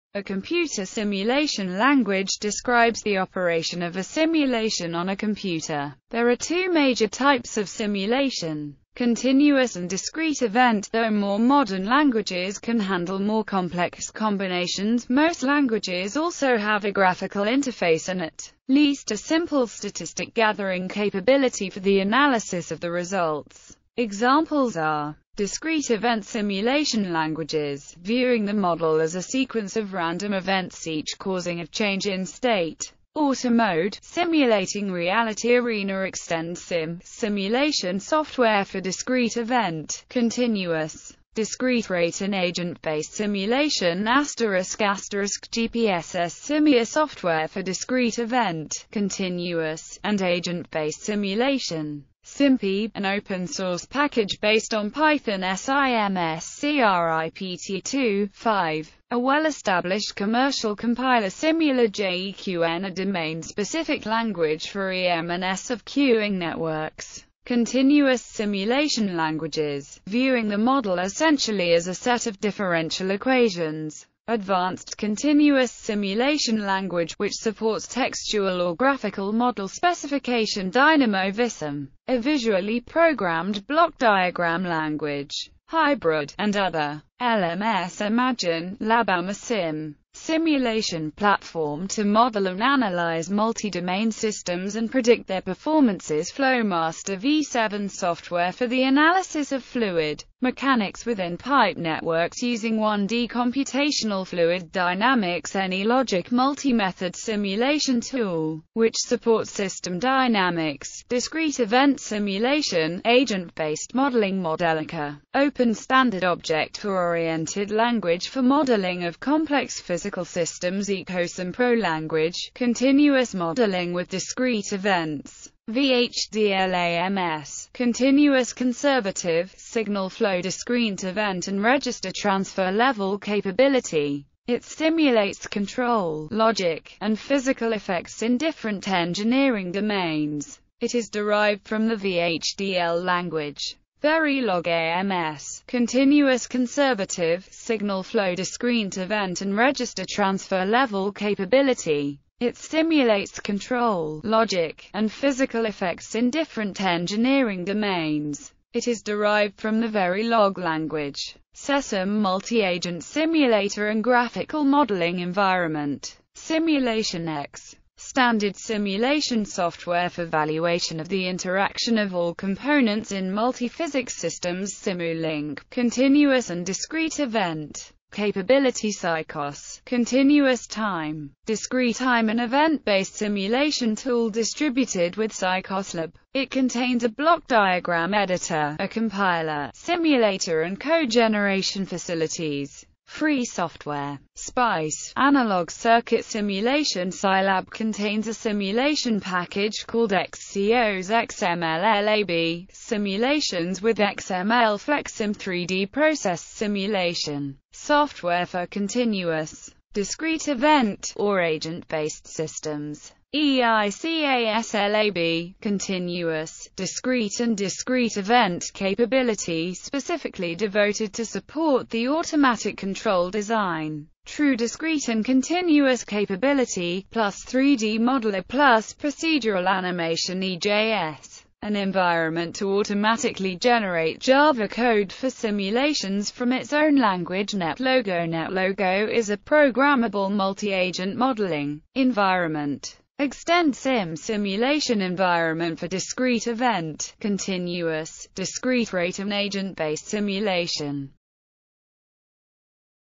Simulation language
A computer simulation language describes the operation of a simulation on a computer.There are two major types of simulation: continuous and discrete event though more modern languages can handle more complex combinations most languages also have a graphical interface and at least a simple statistic gathering capability for the analysis of the results.